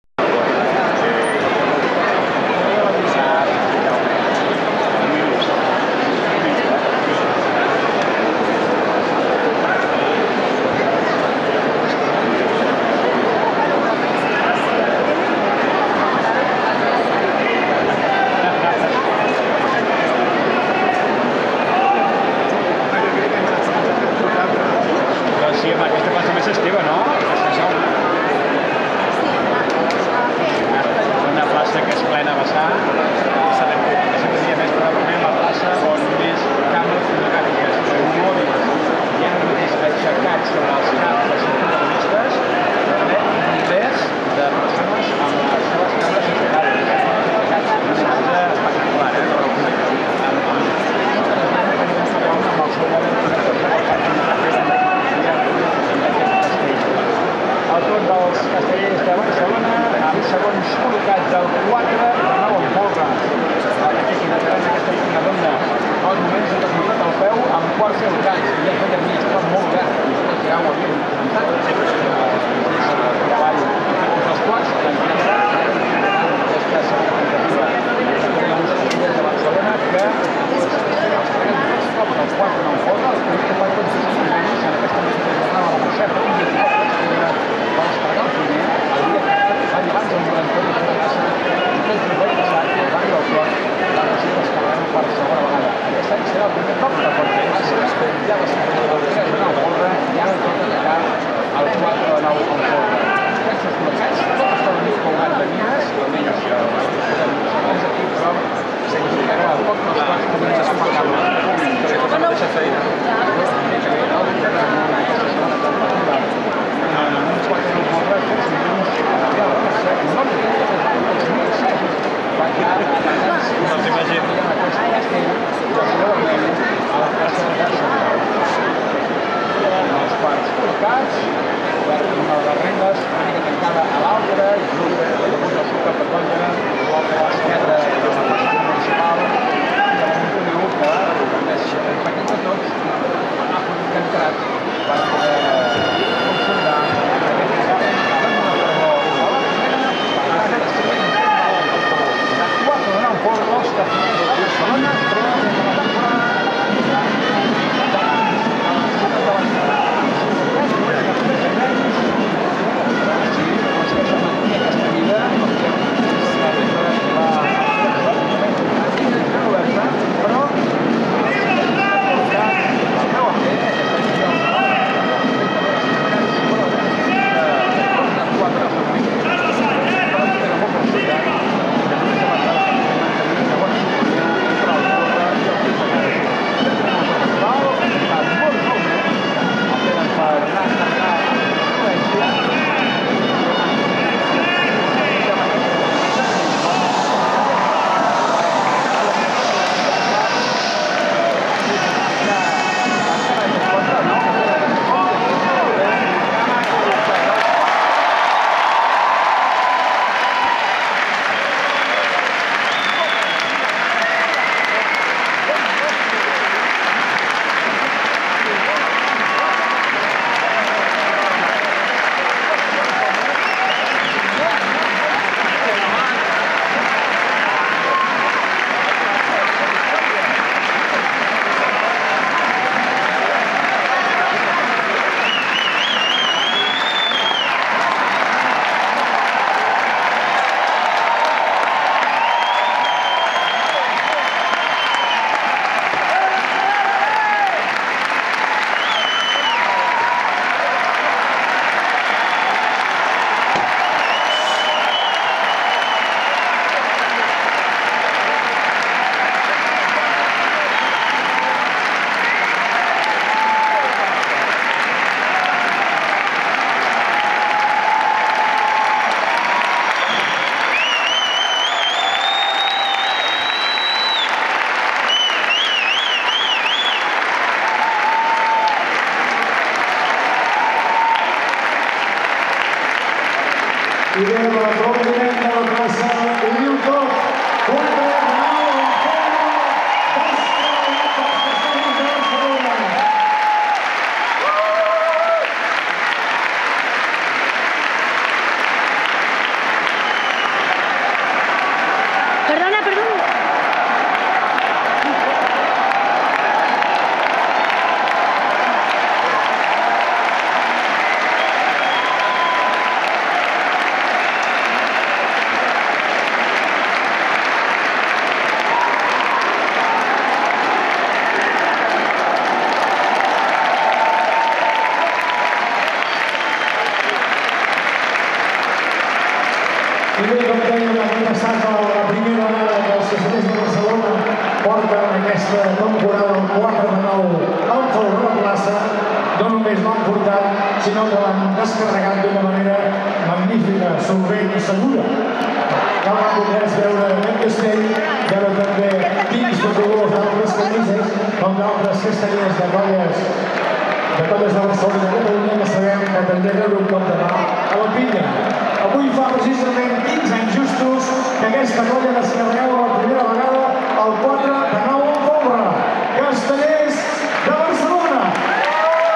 Castellers de Barcelona: 4d9f - Festa Major de Barcelona - La Mercè Colles Convidades 21/09/2014.
Castellers de Barcelona: 4 de 9 amb folre - a la Plaça de Sant Jaume de Barcelona, amb motiu de la Festa Major de Barcelona - La Mercè i diada de Colles Convidades.